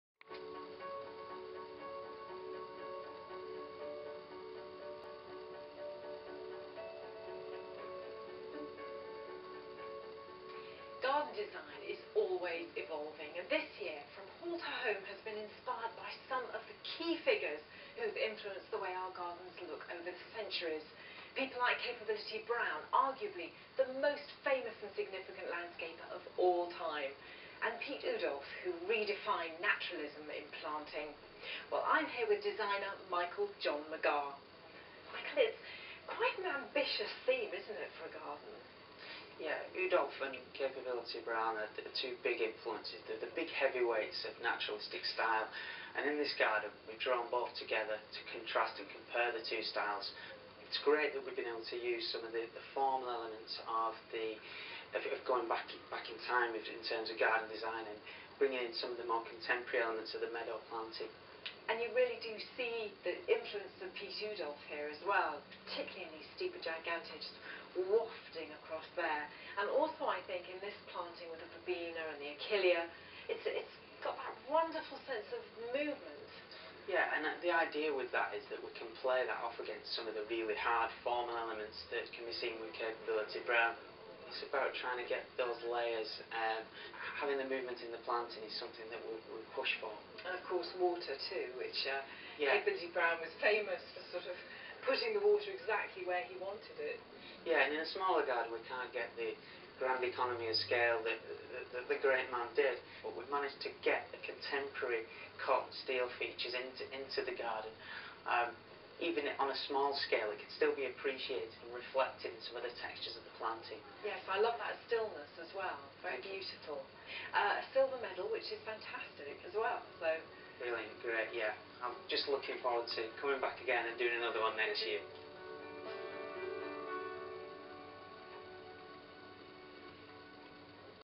Garden designer Michael John McGarr interview at RHS Tatton in Cheshire July 2016
Garden and landscape designer Michael John McGarr is interviewed about his RHS Tatton garden in 2016, in Knutsford, Cheshire. Called 'From Hall to Home', the garden is a contemporary landscape, inspired by the garden designer Capability Brown, drawing in naturalistic planting and water features, with a modern twist.